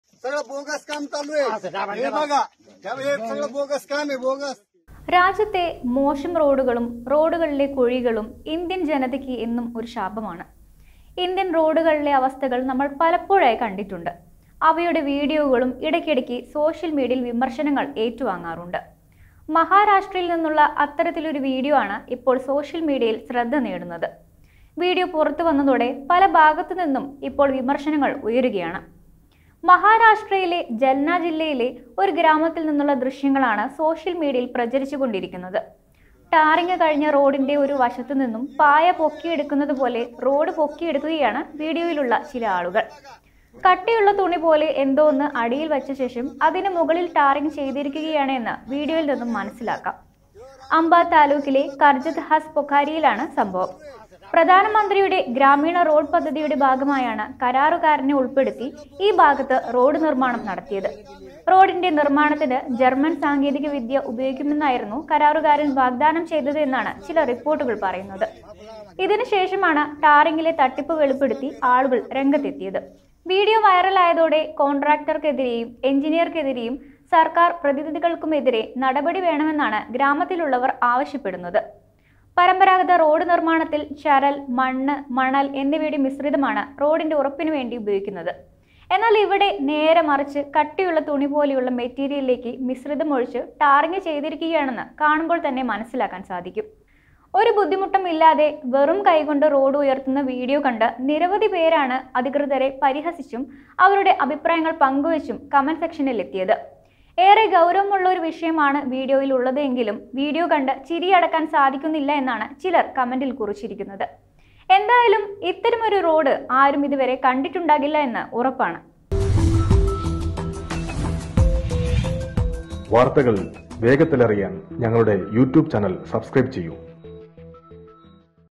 ഈ റോഡ് 'പൊളി'യാണ്, ചുരുട്ടിയെടുക്കാം; മഹാരാഷ്ട്രയില്‍ നിന്നും ഒരു നാണക്കേട് 'മോഡല്‍'|Viral video
ഈ റോഡ് 'പൊളി'യാണ്, ചുരുട്ടിയെടുക്കാം; മഹാരാഷ്ട്രയില്‍ നിന്നും ഒരു നാണക്കേട് 'മോഡല്‍'